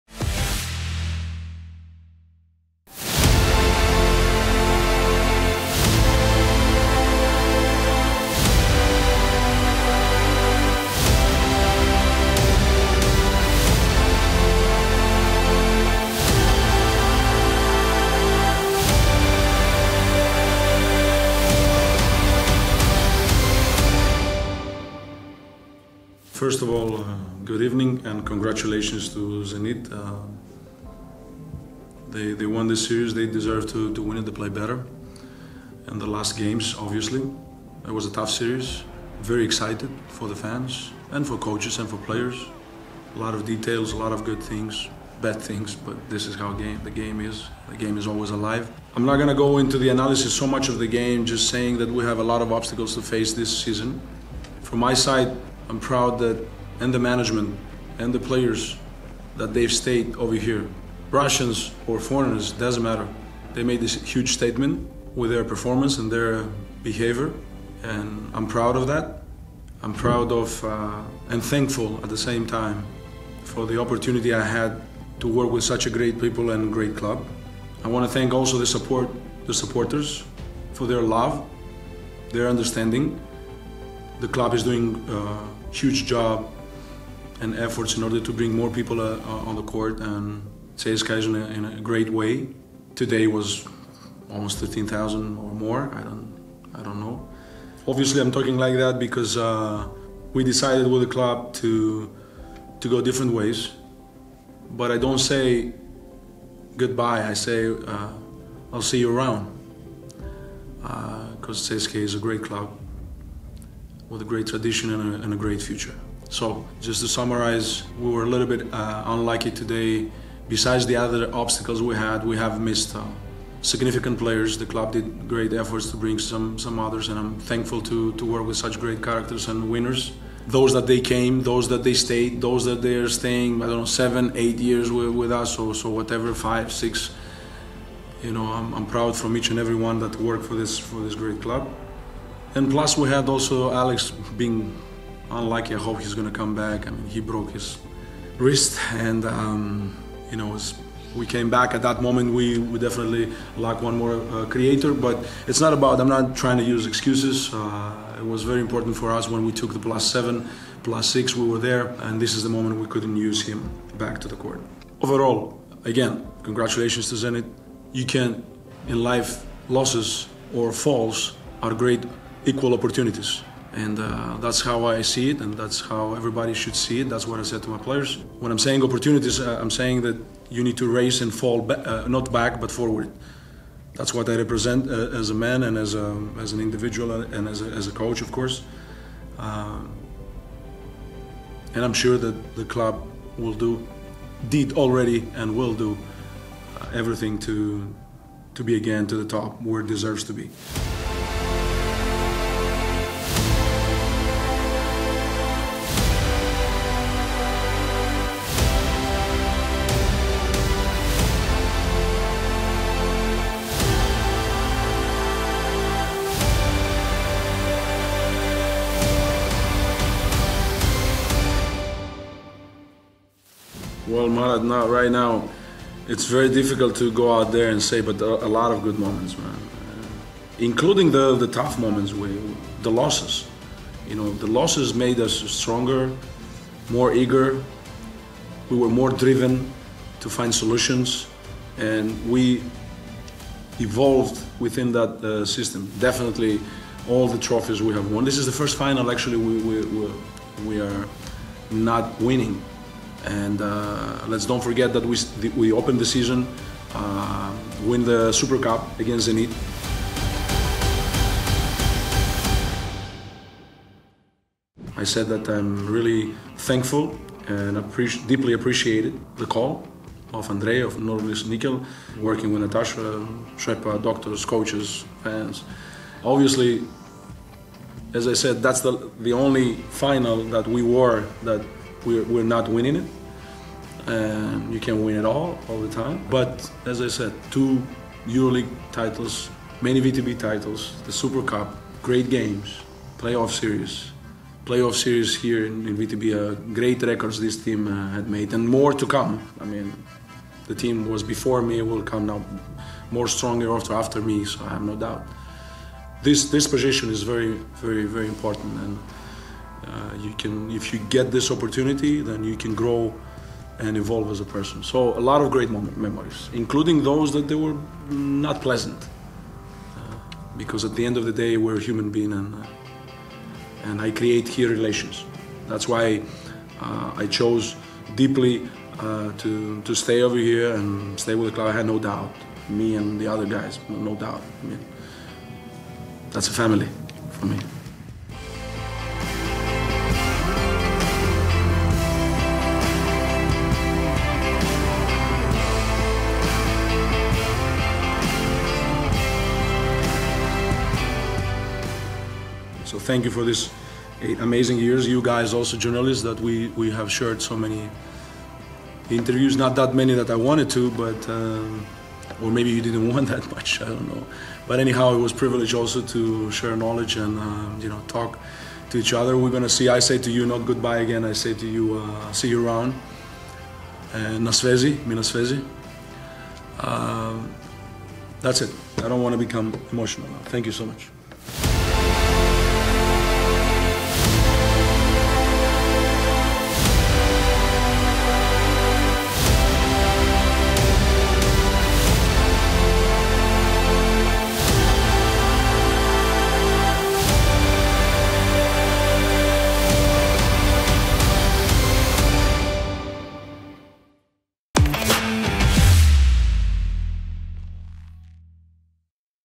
«Это семья для меня». Прощальная пресс-конференция Димитриса Итудиса
Димитрис Итудис прощается с ЦСКА после 8 лет в клубе.